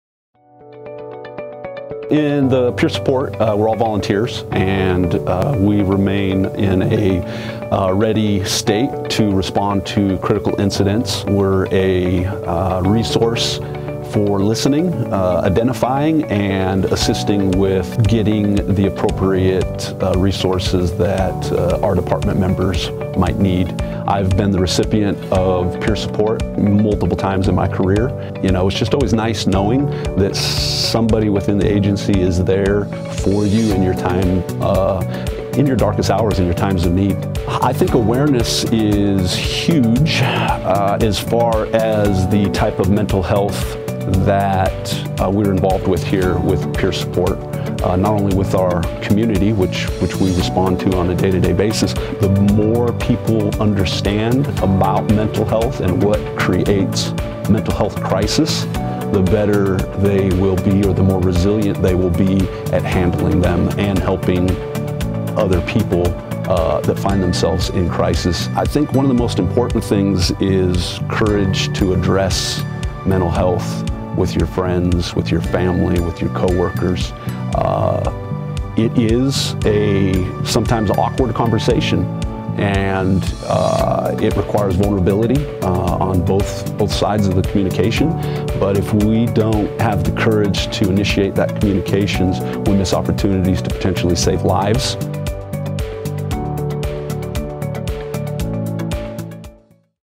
Mental Health Awareness Month 2022
As we close out Mental Health Awareness Month, Lt. Michael Moseley shares how we take care of our own.  The Pima County Sheriff’s Department takes pride in supporting each other and our families.
If you, or somebody you know, is experiencing a mental health crisis there are multiple resources at your disposal. Arizona Complete Health offers a 24/7 behavioral health crisis line at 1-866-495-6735.